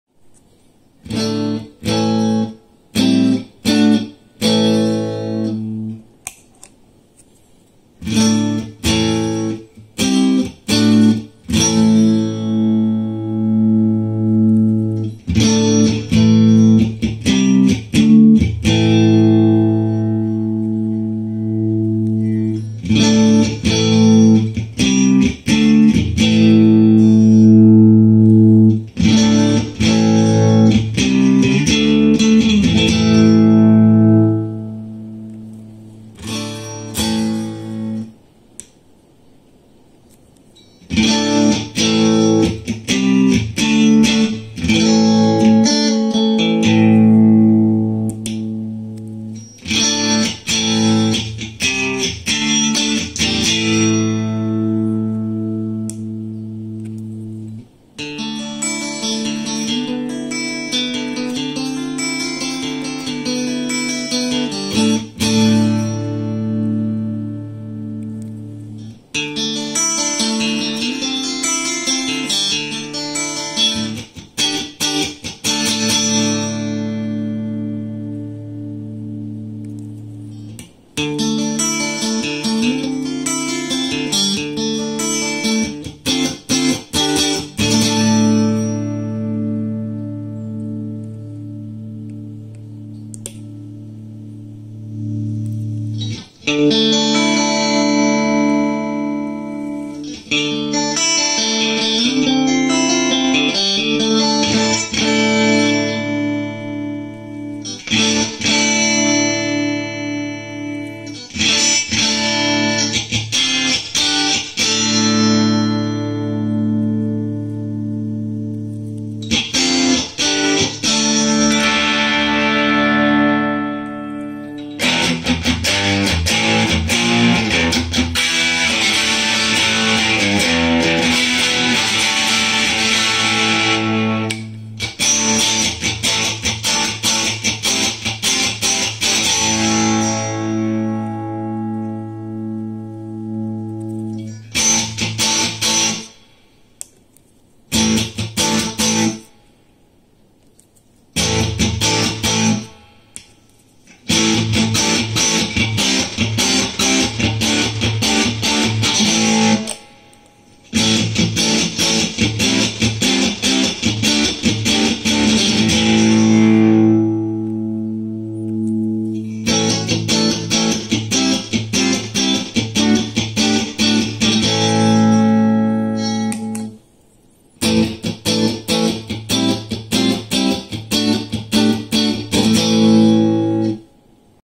Jackrabbit FTvin emulator preamp hand made in Poland
guitar effect based on "fender" amplifier. overdrive handmade in Poland